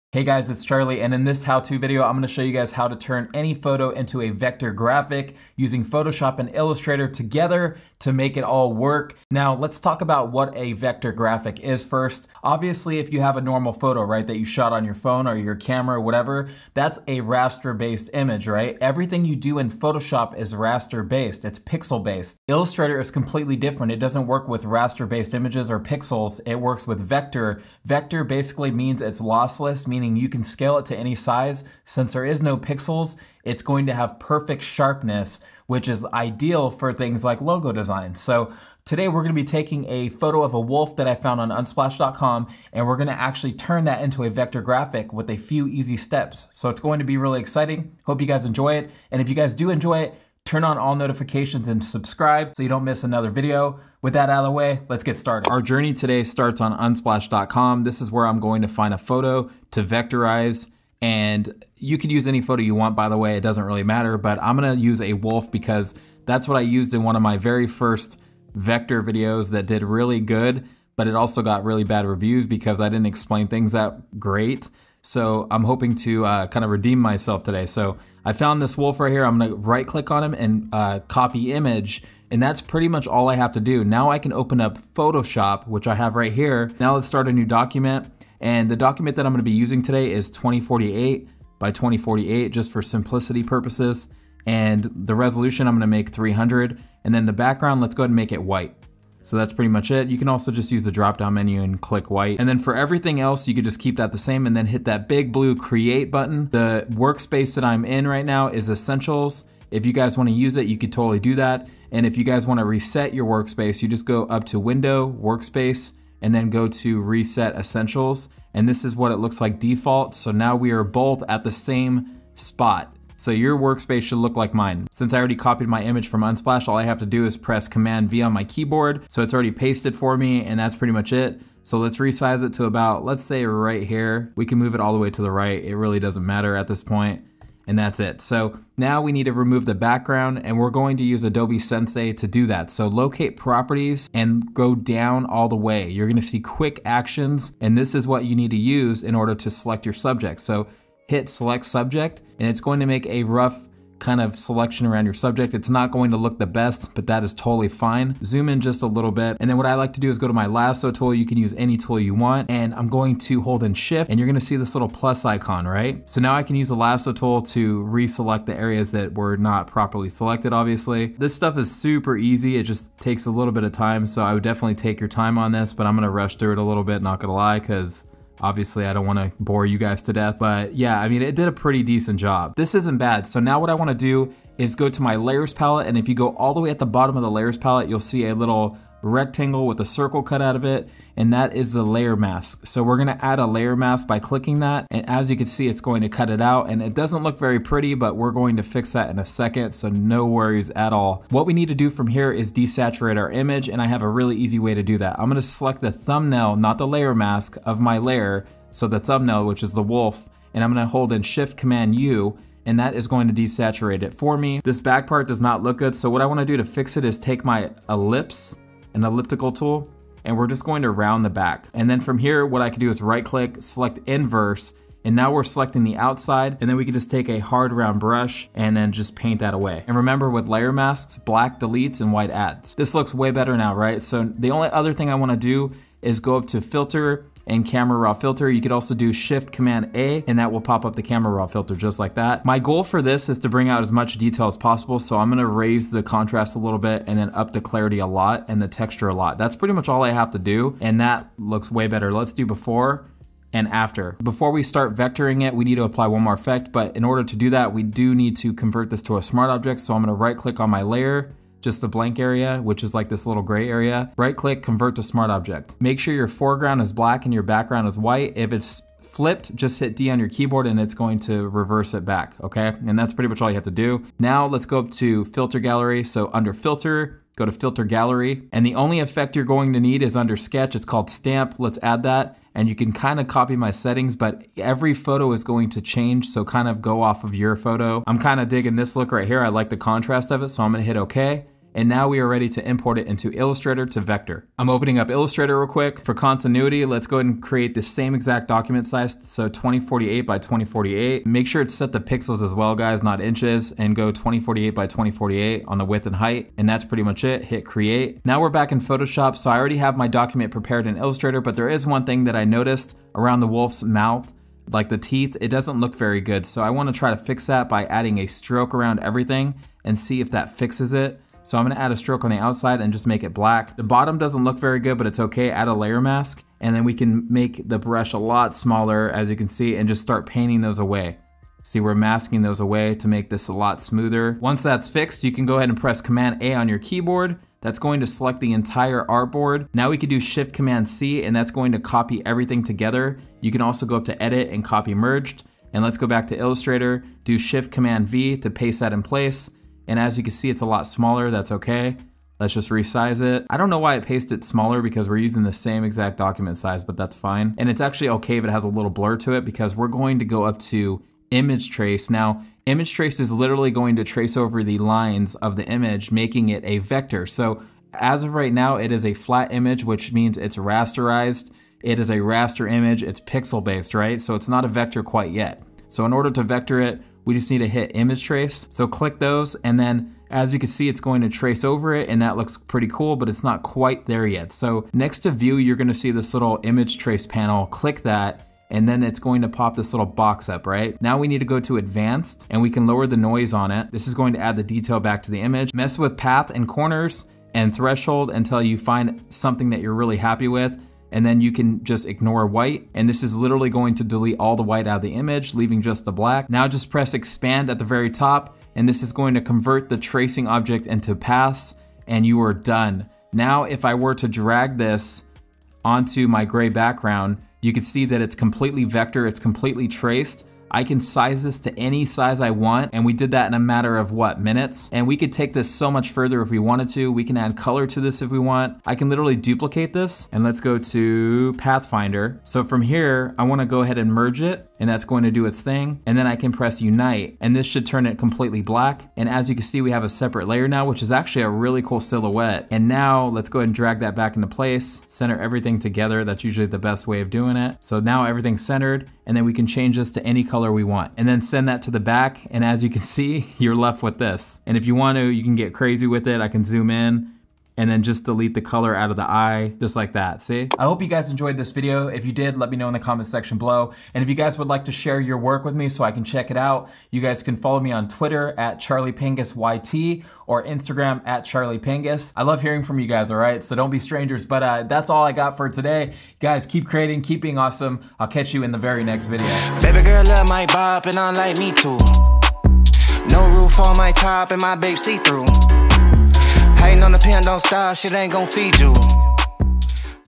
How to VECTORIZE ANYTHING using Adobe Photoshop and Adobe Illustrator
Learn how to vectorize an image using Adobe Photoshop and Adobe Illustrator together! This is a super easy method that even beginners can learn fast. This is a step-by-step tutorial for beginners! Illustrator Tutorial and Photoshop Tutorial Breakdown. If you're a graphic designer and love to learn this channel is for you.

🖥Products I Use Everyday